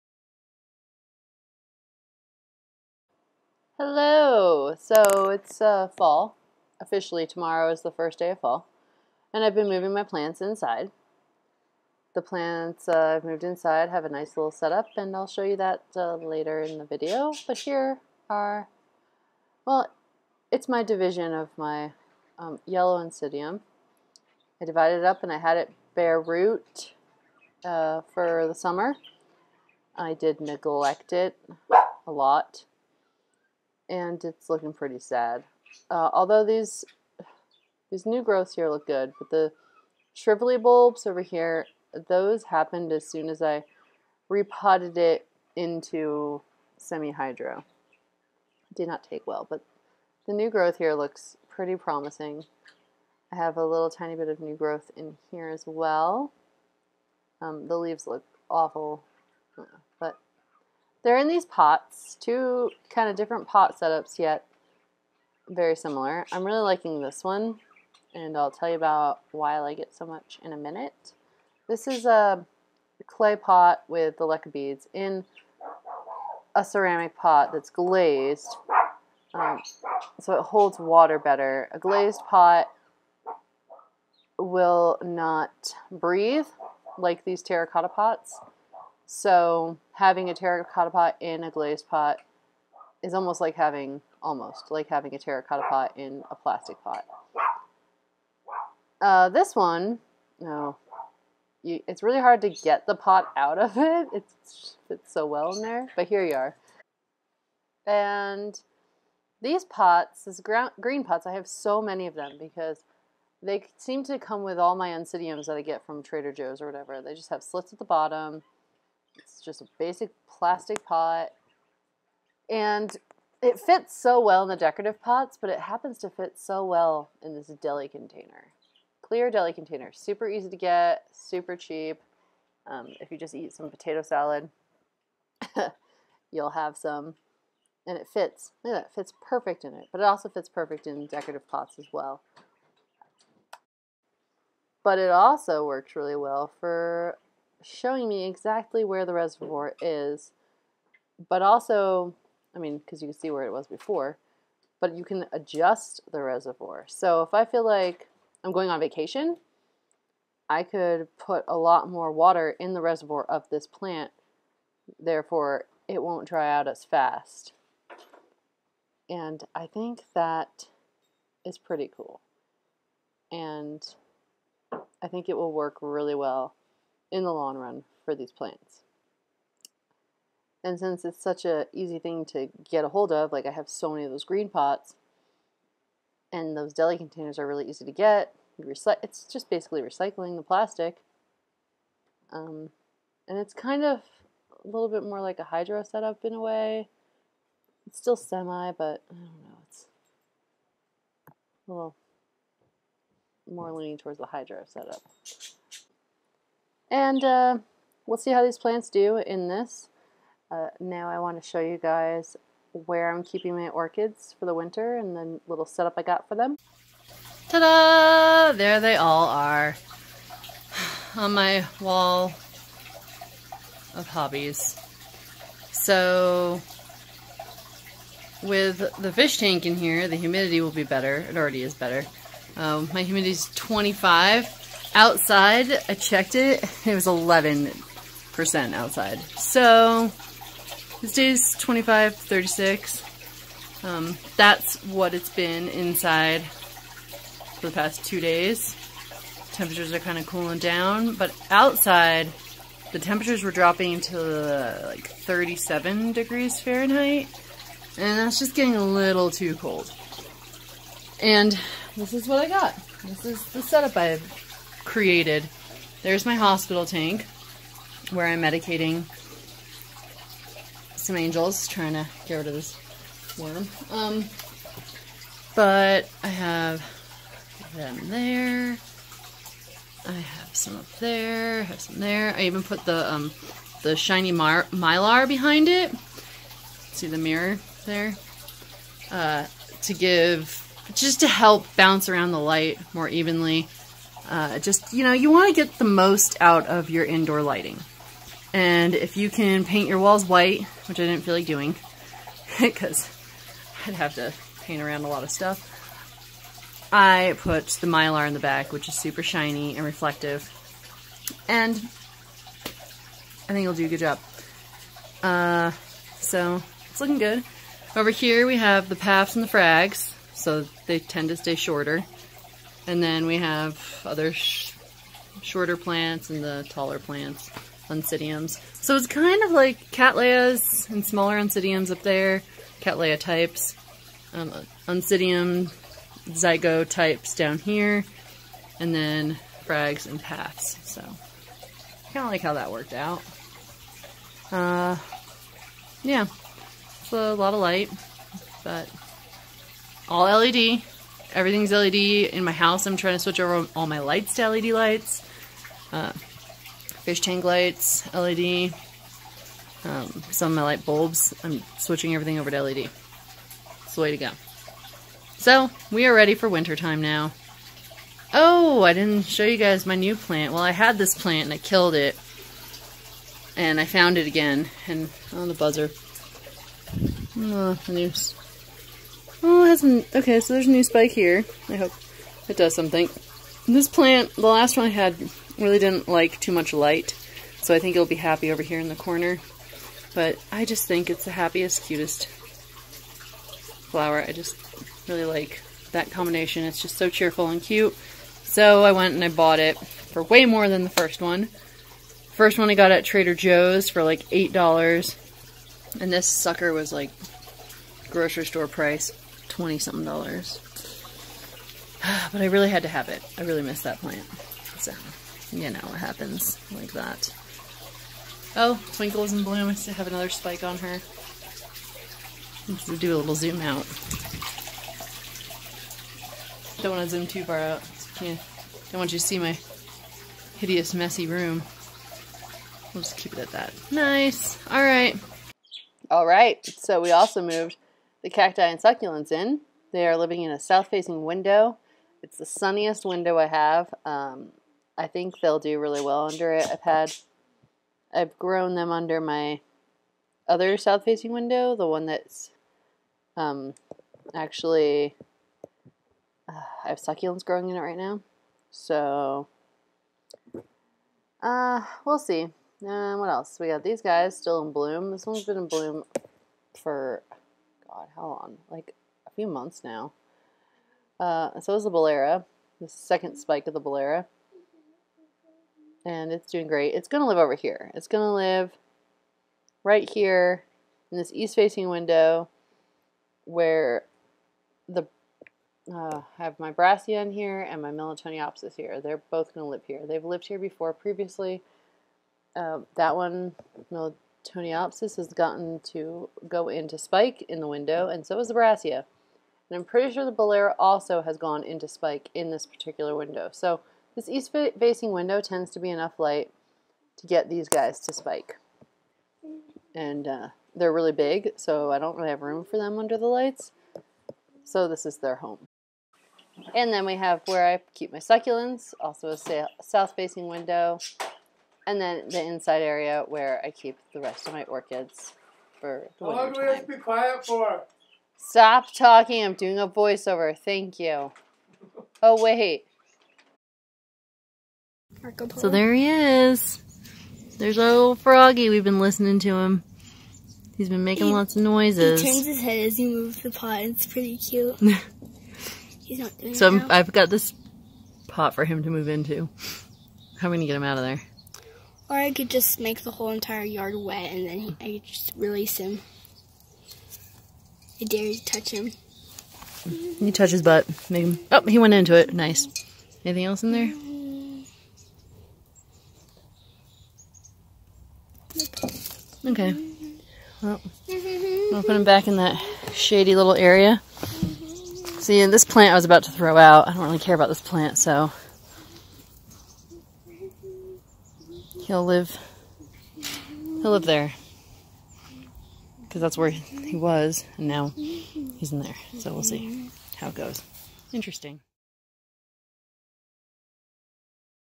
Bring the plants in.
A little update and my indoor grow space.